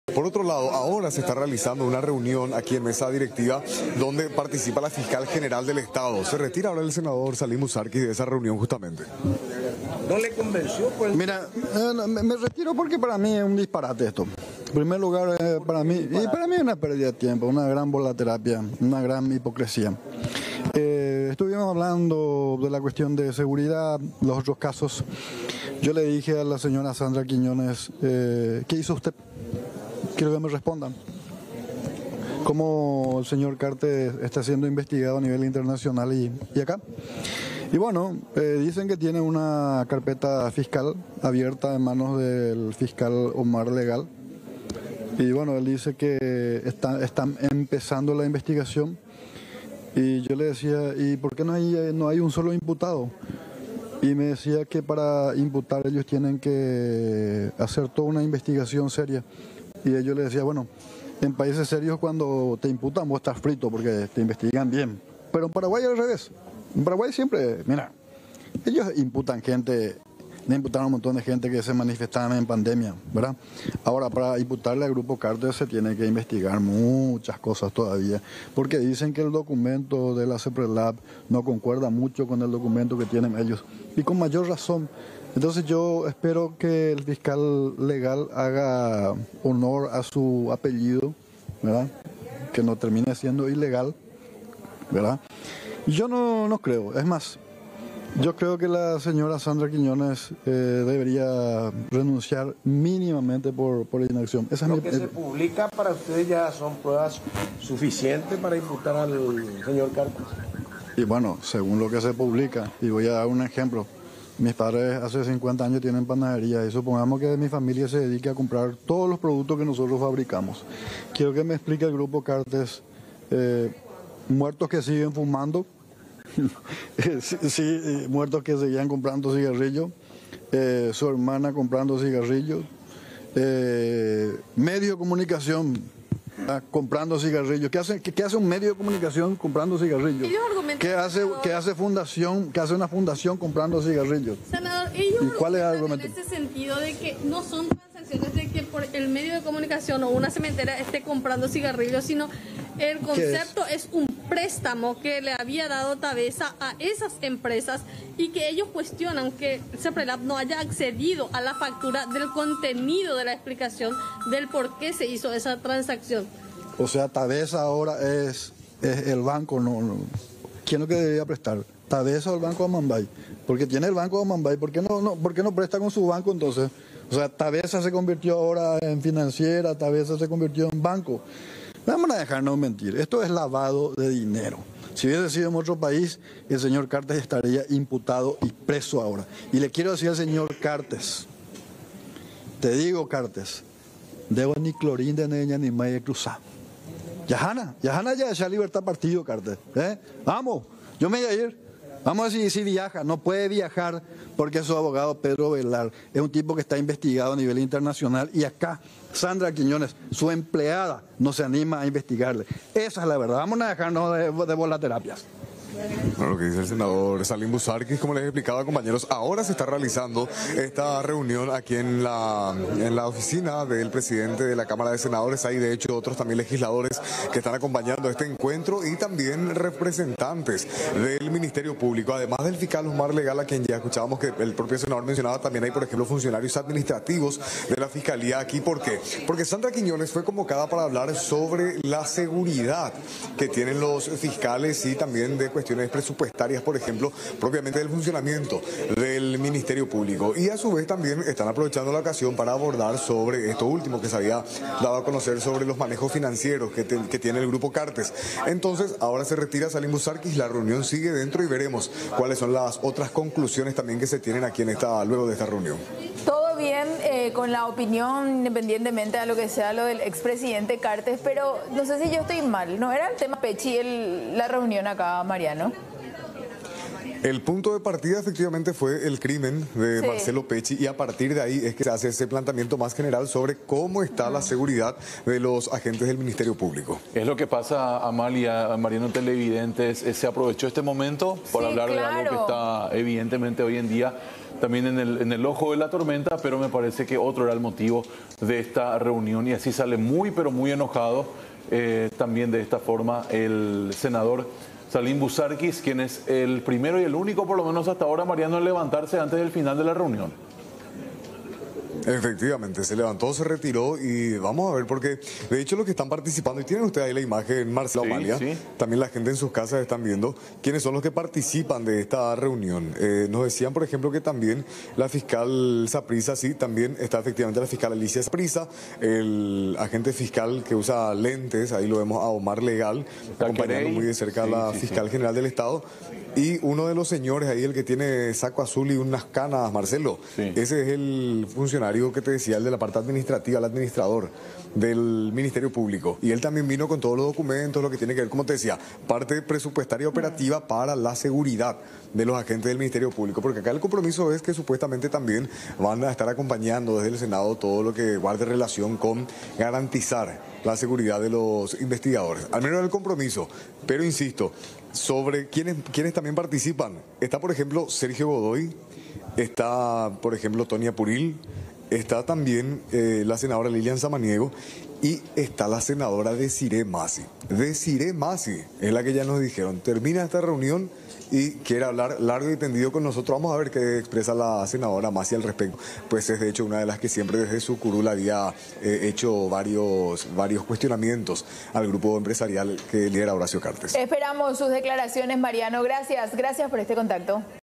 Sandra Quiñonez en la Cámara Alta
Reunión en la cámara alta sobre seguridad a cargo de la Fiscal General del Estado Sandra Quiñonez.

El congresista Enrique Buzarquis se retiró de la reunión muy molesto.